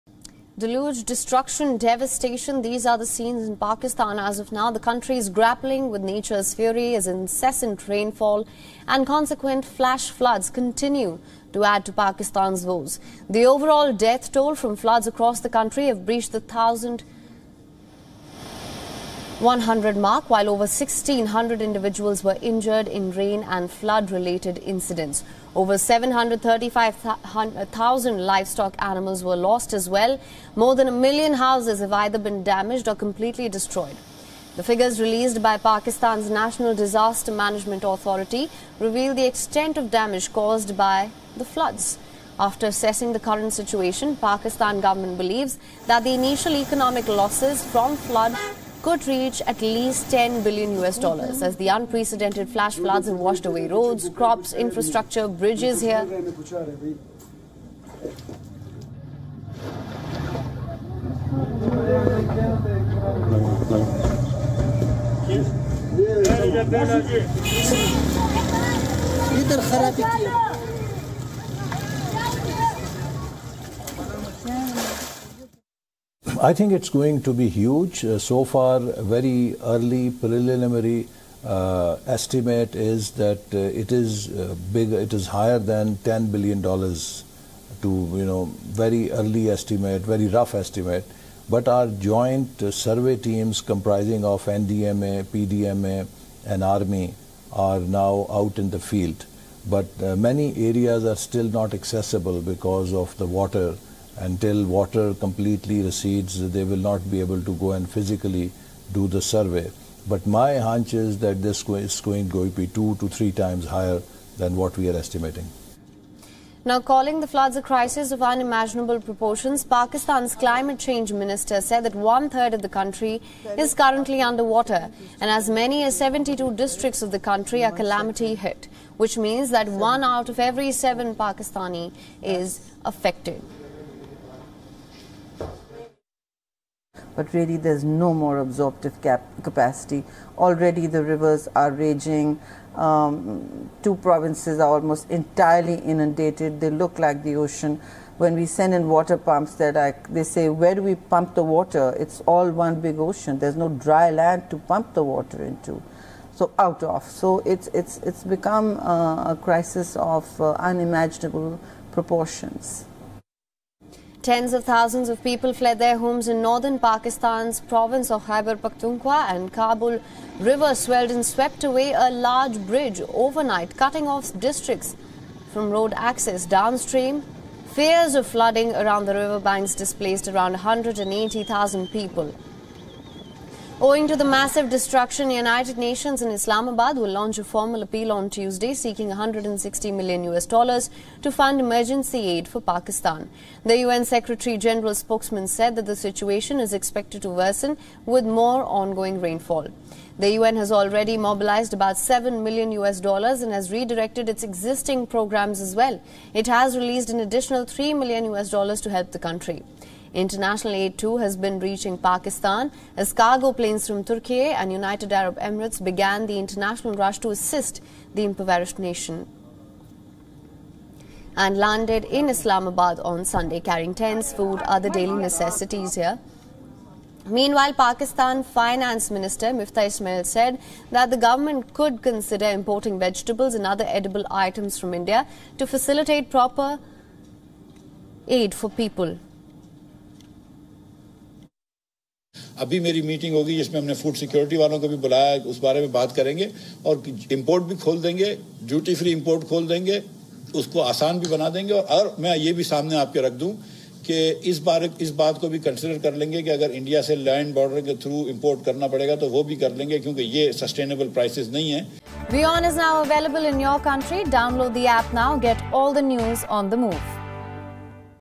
Incessant rainfall and flood batter Pakistan, Shehbaz govt hints at Indian imports amid shortages
Pakistan is currently grappling with nature's fury as incessant rainfall and  consequent flash floods continue to add to the already existing woes.